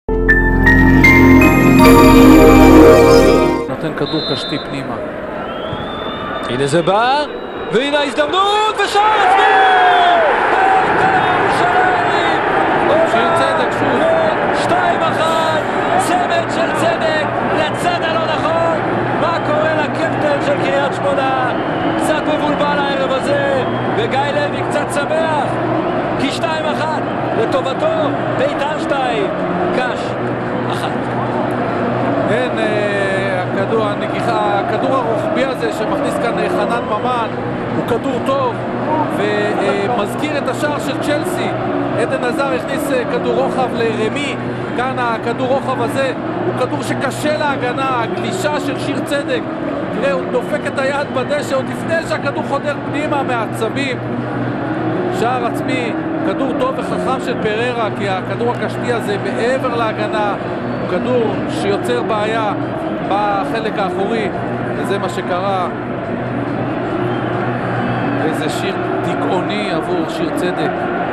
בית"ר ירושלים - עירוני קריית שמונה 1-2 השער העצמי השני של שיר צדק
שיר צדק הבקיע שוב שער עצמי. קלאודמיר פריירה הרים כדור לתוך הרחבה, חנן ממן העביר רוחב ושיר צדק הכניס את הכדור לתוך השער